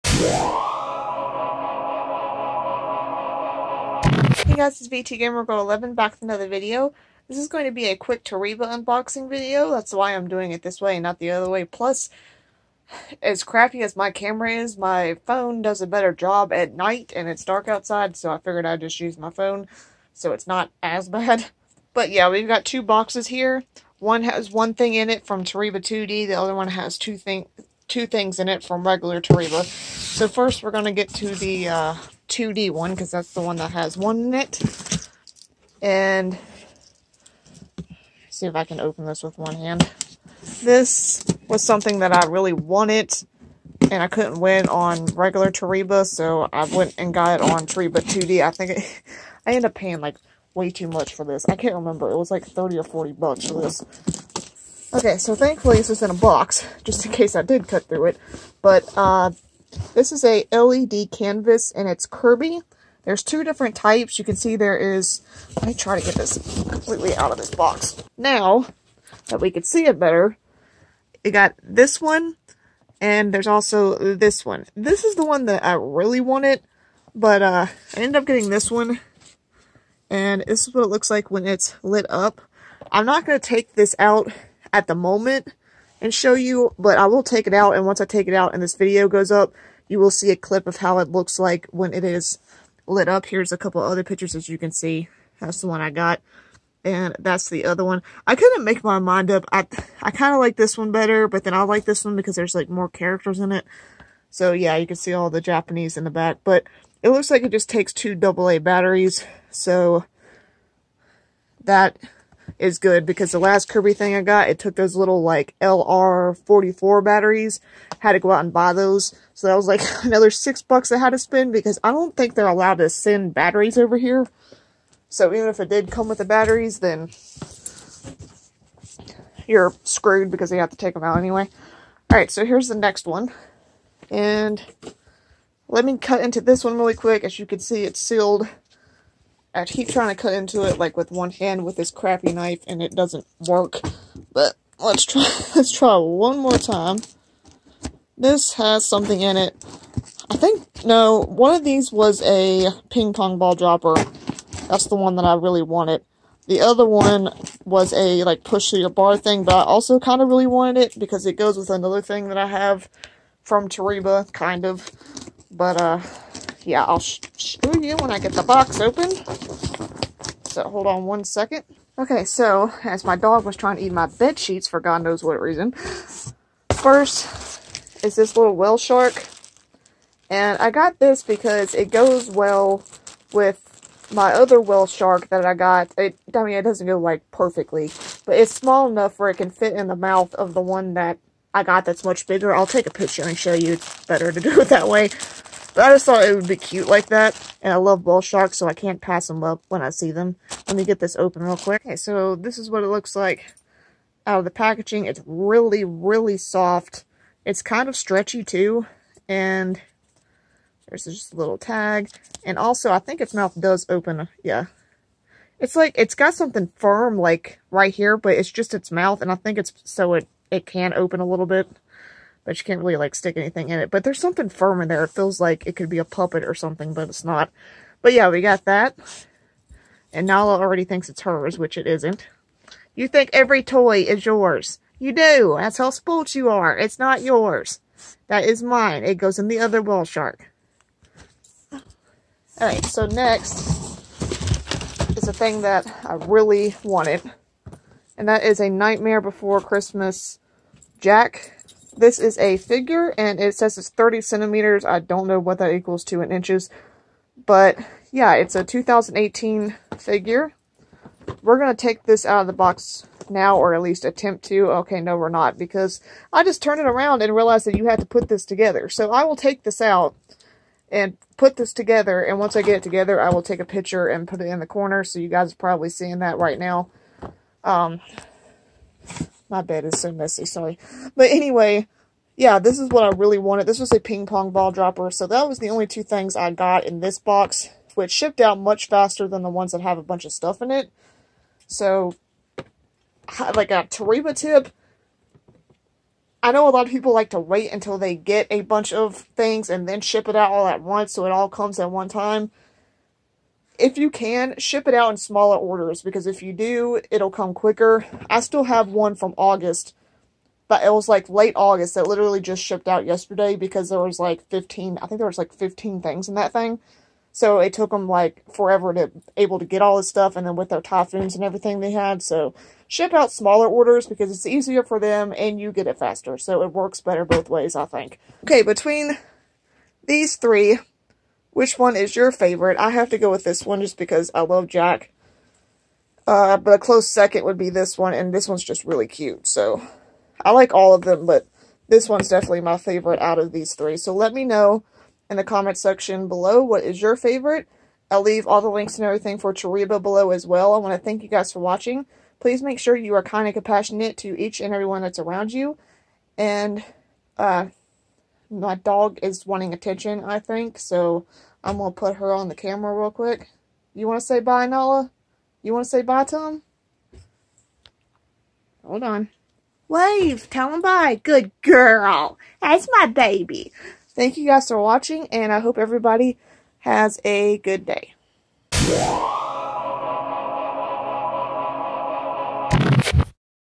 Toreba Unboxing - Kirby and Jack Skellington + A Whale Shark
What is your favorite item I won in this video?

Sorry for forgetting to put the winning clip of the Jack figure in this video, but it was a normal ping pong dropper win.

"A chain reaction of kindness, no matter how small, is ever wasted."
- Rachel Joy Scott

"Don't care about what other people think of you. There will always be those people who want to see you fail because they can't succeed!"

"Haters are going to hate and karma is going to wait!":)